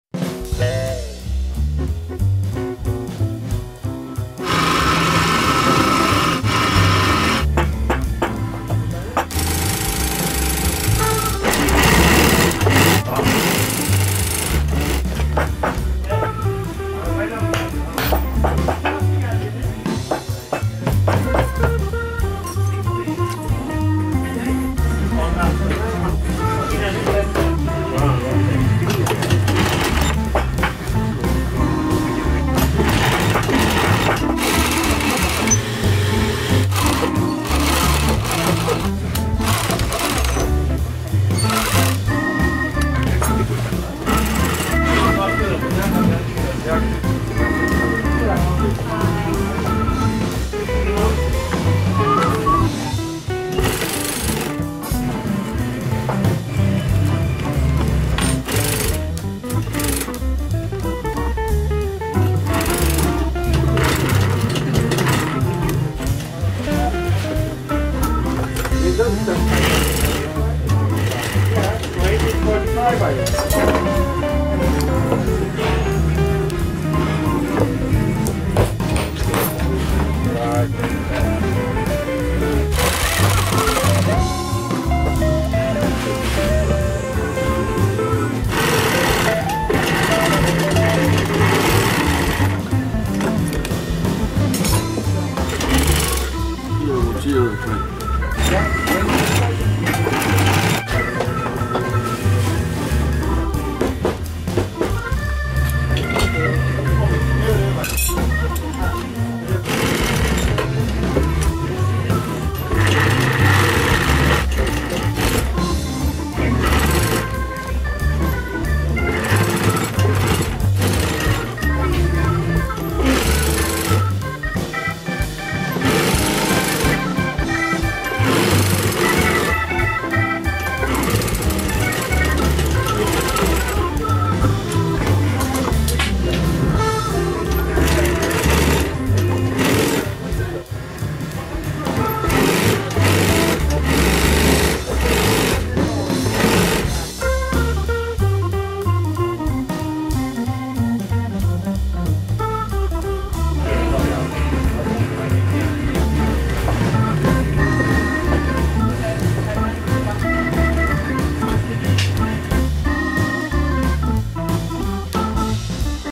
The making of a backpack @Village Tannery
Understanding your equipment and its capabilities to take them to the extreme!!!
This video is the steps that it takes in making a backpack